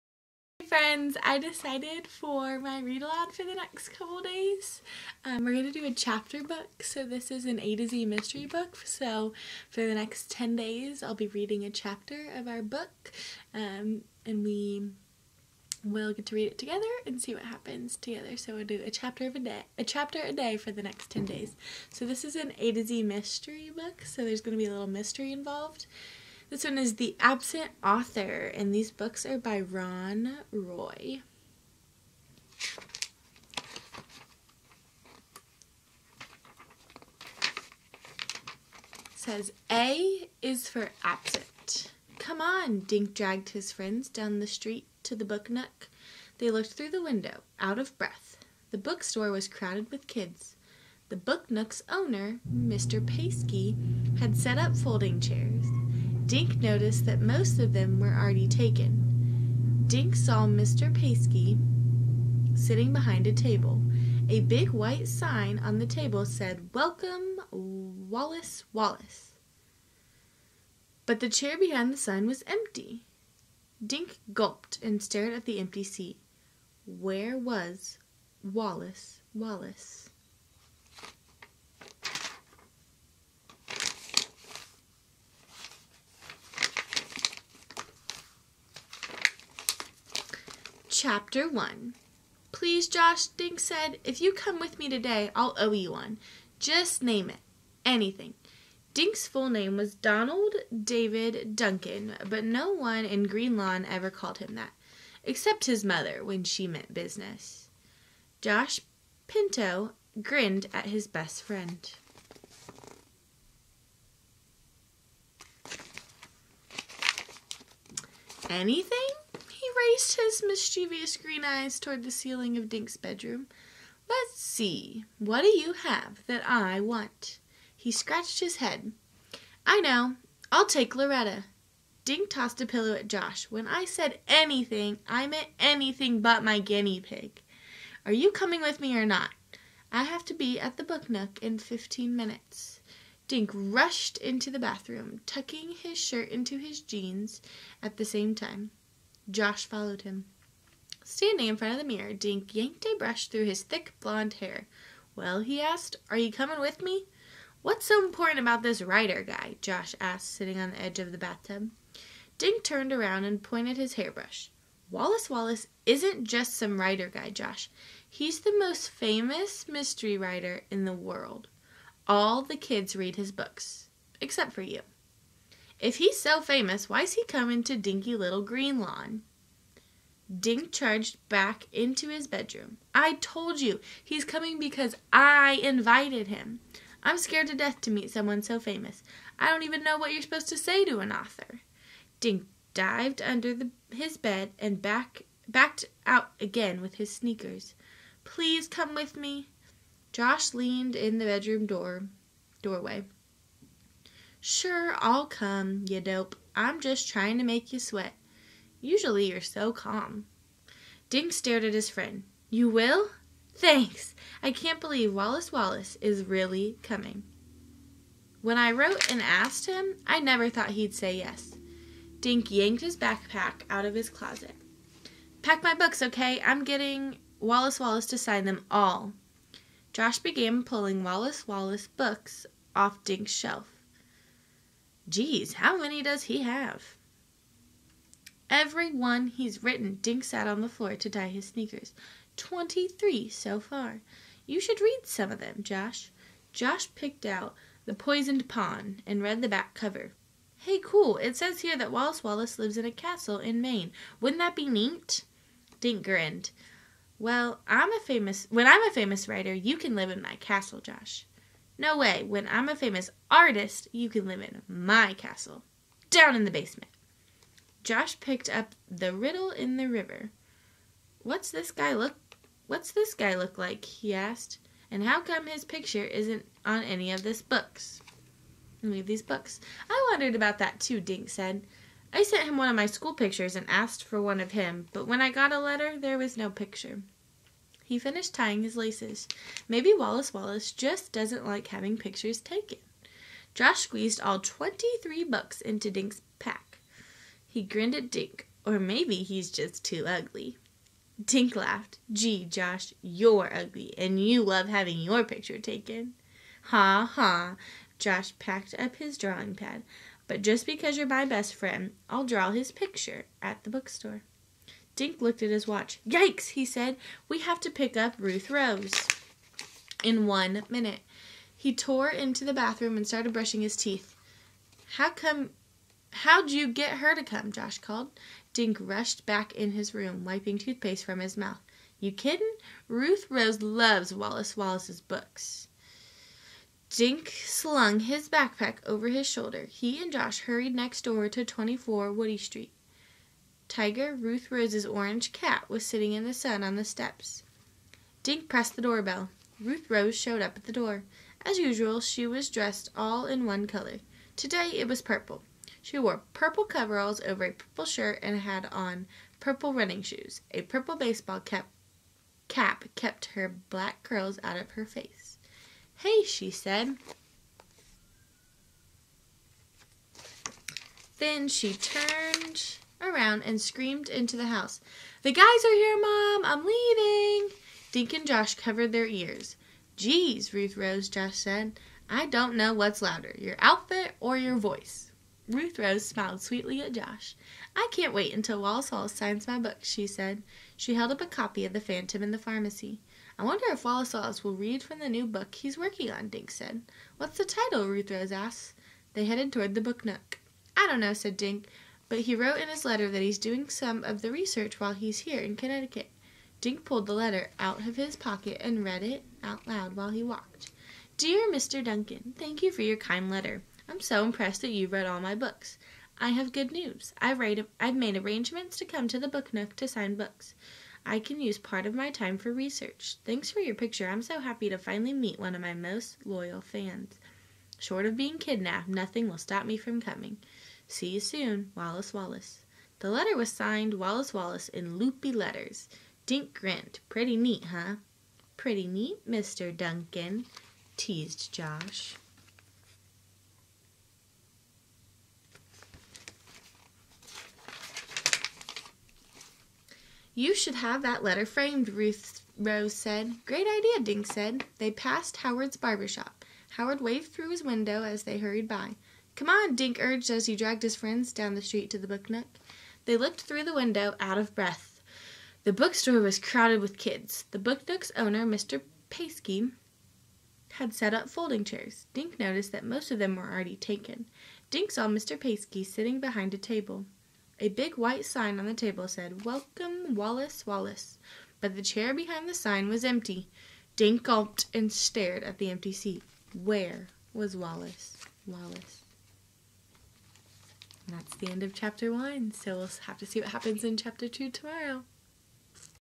Miss Drake's Read Aloud: A to Z Mysteries "The Absent Author" CHAPTER 1
A to Z Mysteries "The Absent Author" by Ron Roy
CHAPTER 1